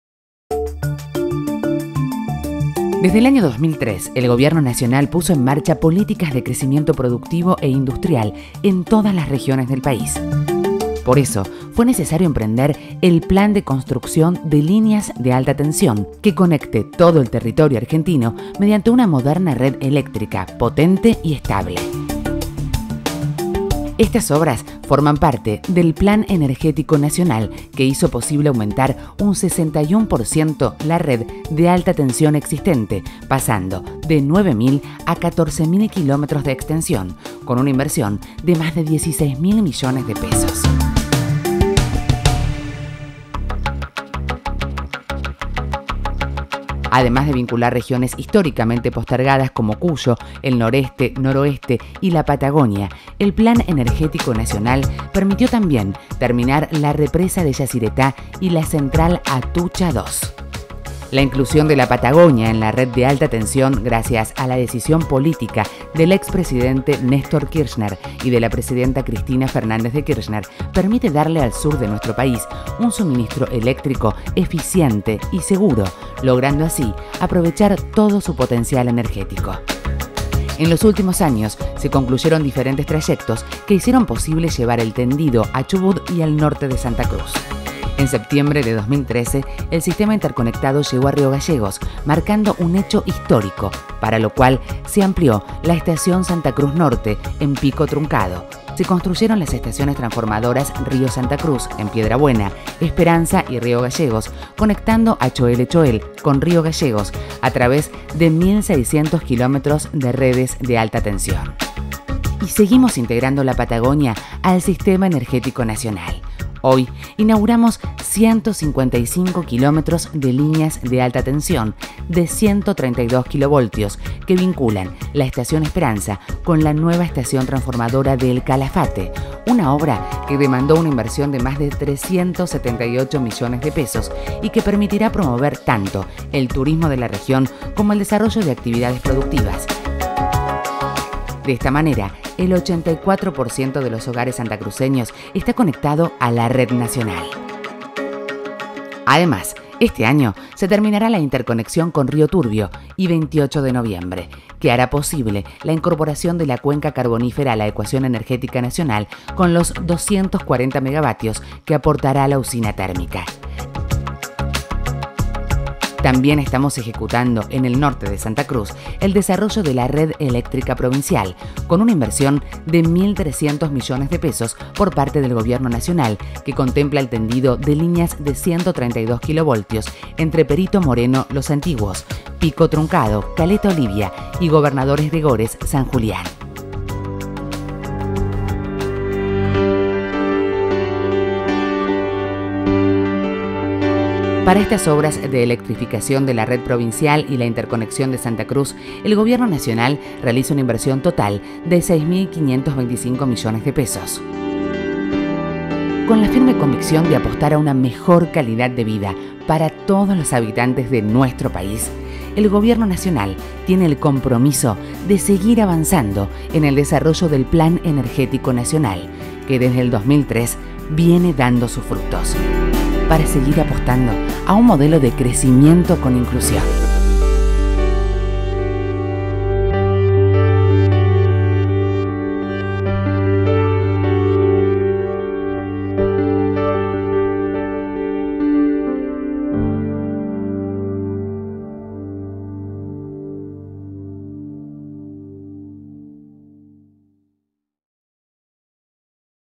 Interconectado de la Patagonia: Línea de 132 kv Esperanza-El Calafate 15 02 2014
La presidenta Cristina Fernández de Kirchner inauguró la vinculación de la localidad de El Calafate al Sistema Argentino de Interconexión. Esta obra es la penúltima etapa de la Interconexión de la Patagonia, la que permite que toda la región sur de nuestro país se haya incorporado al Sistema de Transporte Eléctrico Argentino.